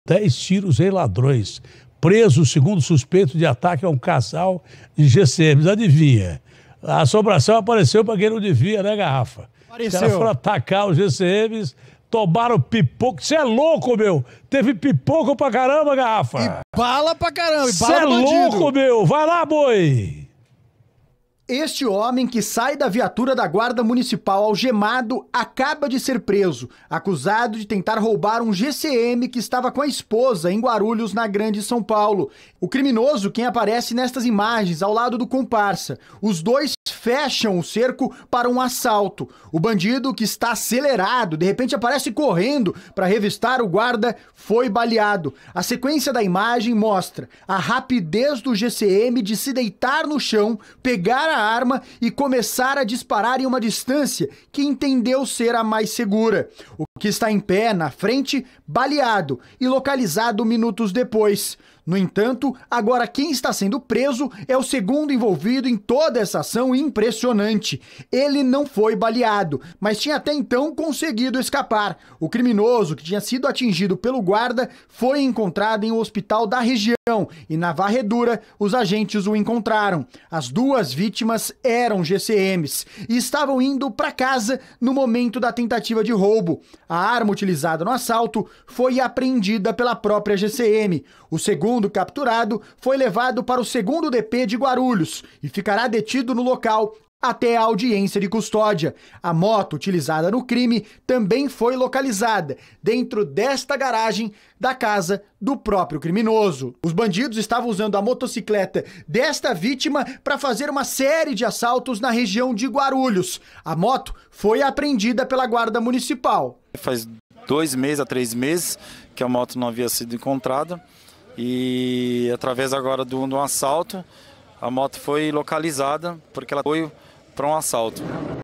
Polícia prende ladrão que atirou contra GCMs
Polícia prende ladrão que atirou contra GCMs em Guarulhos.